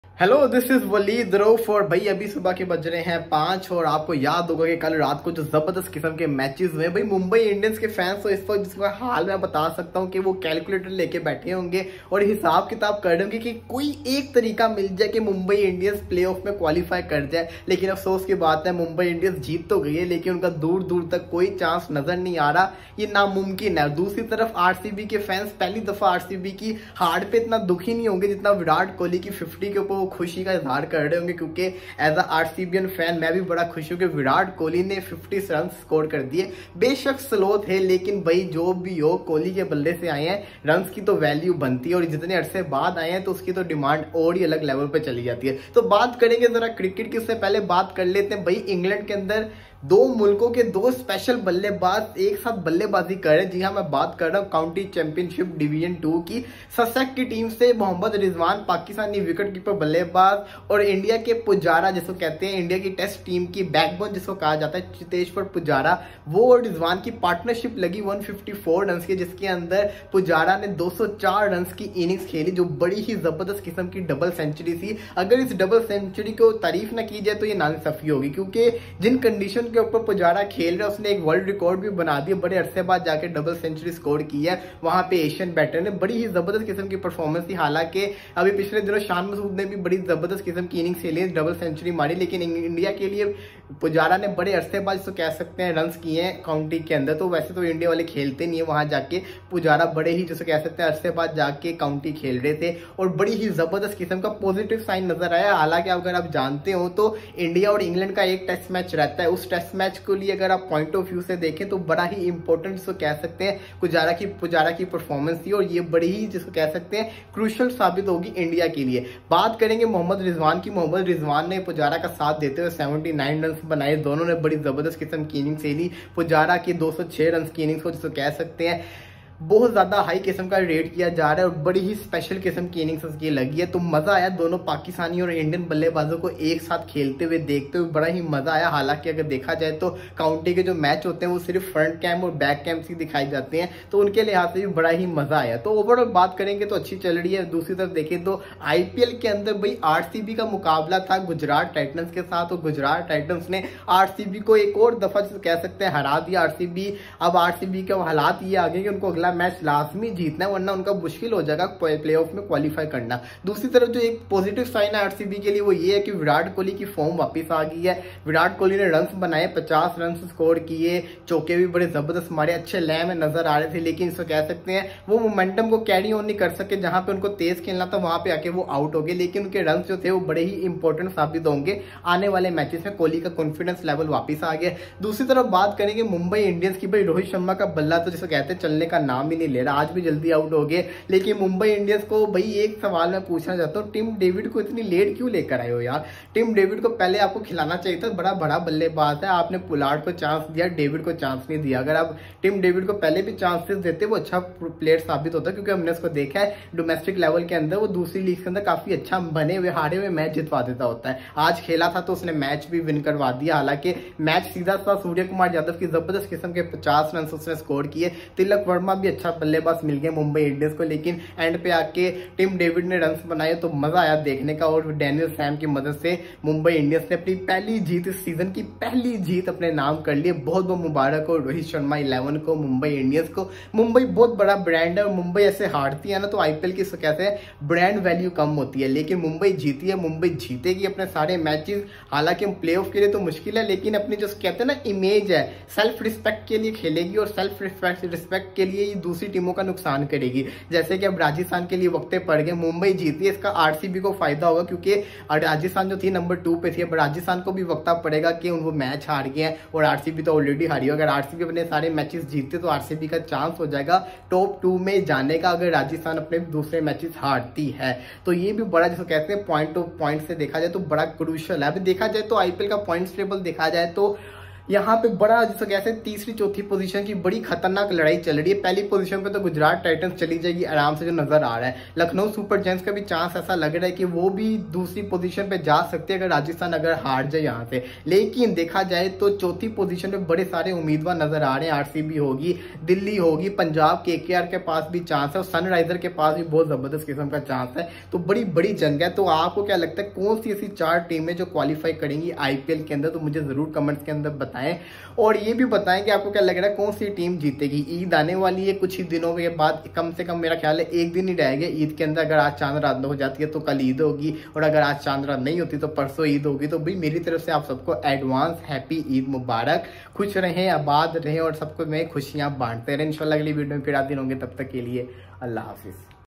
Kohli ne maari 50 || Pujara & Rizwan dream partnership || Mumbai wins Pakistan reaction on IPL 2022
My reaction on Ipl 2022
Matches
RCB vs GT and MI vs RR
In this video you’ll see my deep analysis on these two matches with that I am discussing dream partnership of Rizwan and Pujara in County Championship division 2
So must watch this video fully

Thankyou so much for watching our Video

 (Pakistan's Reaction)